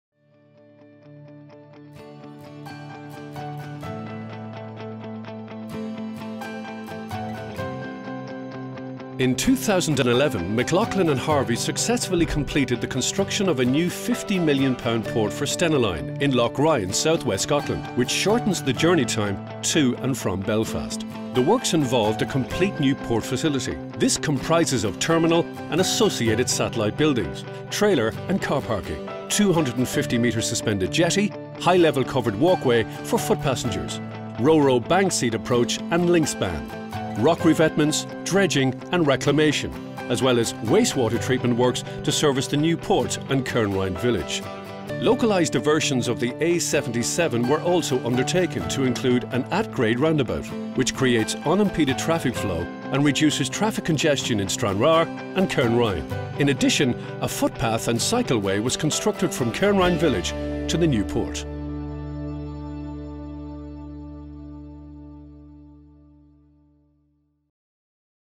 Loch Ryan Terminal: Introduction & Rock Armour
First of five videos documenting the project and the challenges of rock armouring are discussed.

Construction of a new Stena Line terminal and port facility.

The terminal comprises of a steel frame 3-storey 21,528sqft with mezzanine floor, and satellite buildings, including security building, workshop, check-in booths, pump house, car and coach search building, sewage treatment plant, and offices.

The port facility involved the construction of rock revetment, dredging, reclamation, flexible bituminous pavement (internal heavy duty roads, marshalling area, carparking), block paving lorry parking, design & construction of 220m x 18m suspended jetty, new terminal building and satellite buildings, realignment of A77 trunk road and construction of an at-grade roundabout.